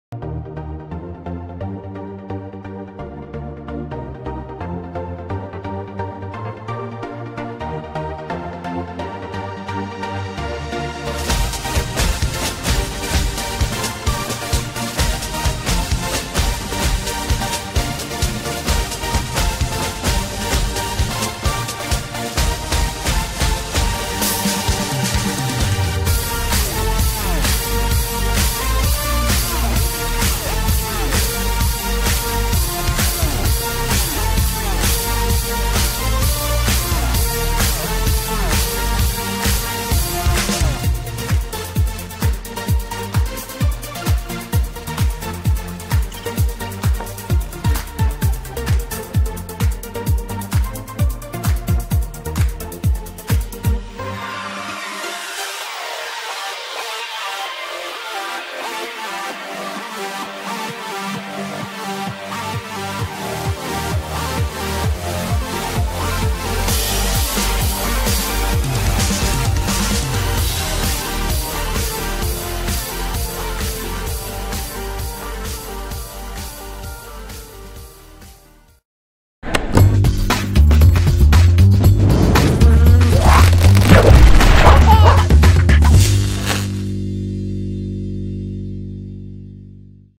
How to turn an old chair into abstract art
Watch how this person throws buckets of paint on an old chair, turning it into an abstract, functional work of art for the studio!